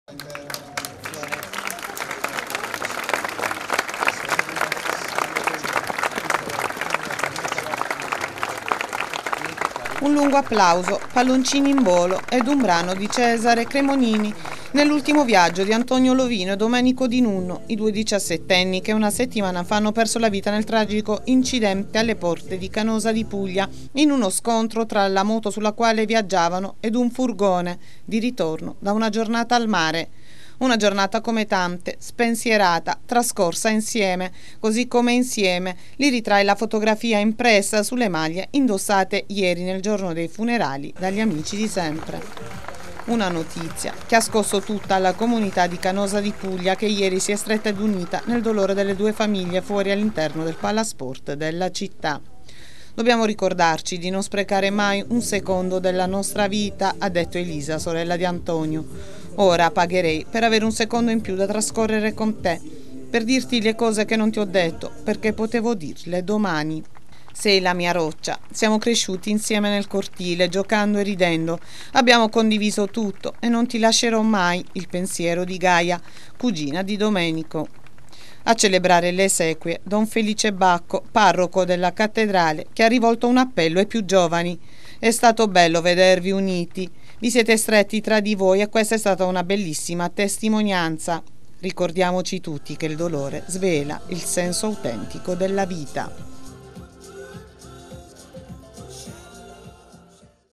Canosa di Puglia - Antonio e Domenico, l'abbraccio della città nel giorno dei funerali
Guarda le videonews e gli approfondimenti alle notizie sul sito - il portale di informazione della Città di Barletta.